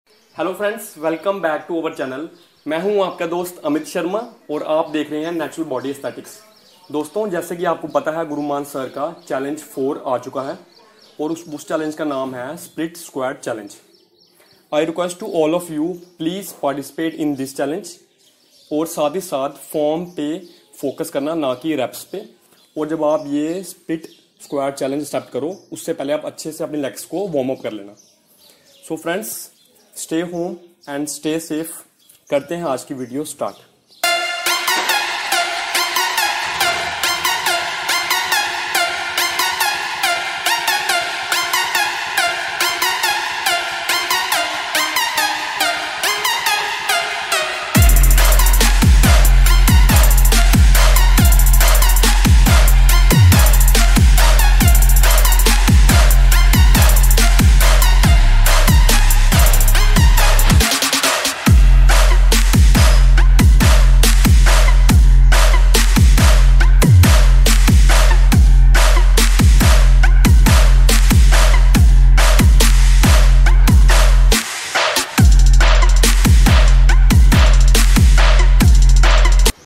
37 Reps - Amit Sharma - Guru Mann Split Squat Challenge 2020
Natural Body Aesthetics accepts Guru Mann's Split Squat Challenge and hits a record of non-stop 37 reps in 45 seconds...
Let's see who wins.